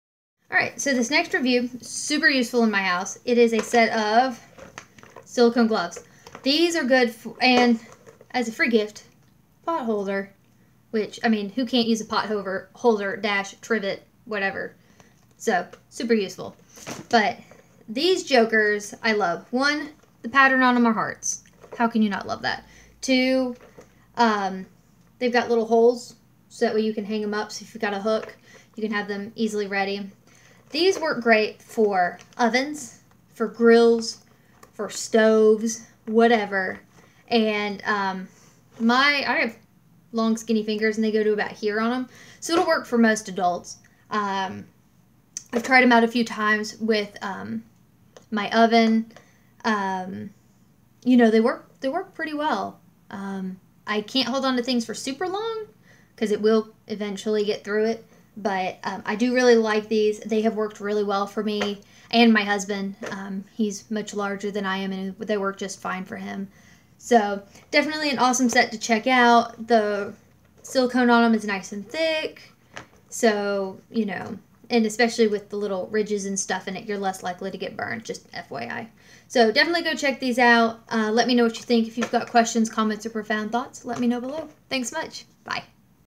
Heat Resistant Silicone Gloves and Pot Holder Review
These gloves work really well for getting hot pans into and out of the oven as well as the grill. I find this works best for baked potatoes and other foil wrapped items. Full disclosure, I received this for free in exchange for my honest review.